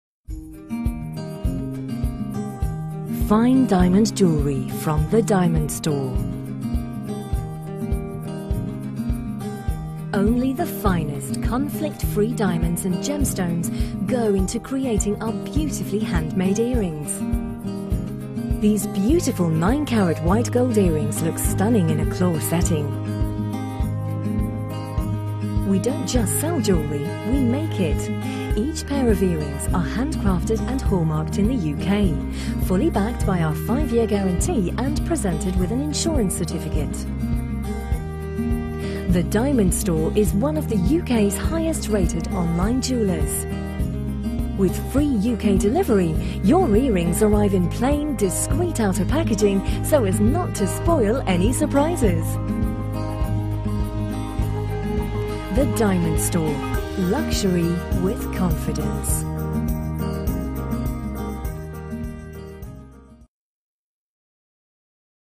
H4516 - GARNET PINK SAPPHIRE AND DIAMOND STELLATO EARRINGS IN 9K WHITE GOLD
Ideal as an elegant birthday, anniversary or Christmas gift. The lucky recipient of these stunning vintage-inspired gem earrings is sure to cherish them. They are crafted with deep red garnets, pink sapphires and diamonds in an ornate lace pattern. A setting of fine white gold adds a substantial, luxurious finishing touch